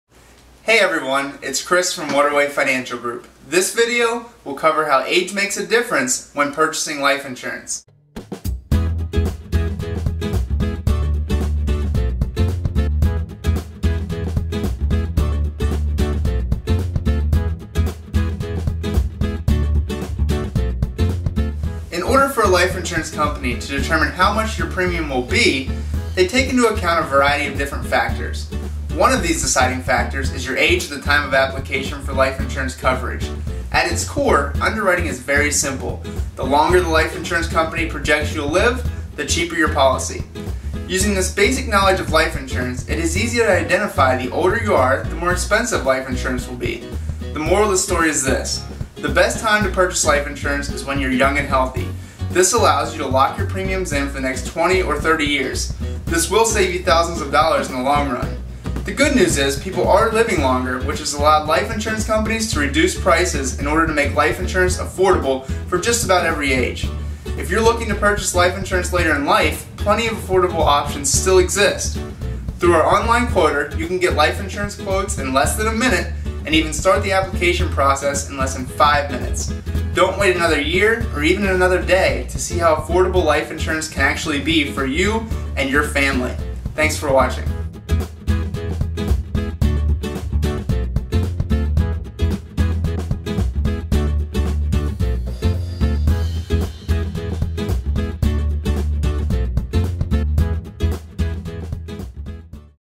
Term Life Insurance | Age Makes A Difference | Waterway Financial Group
Age makes a difference when applying for term life insurance, and it is important to understand what to expect when searching for rates.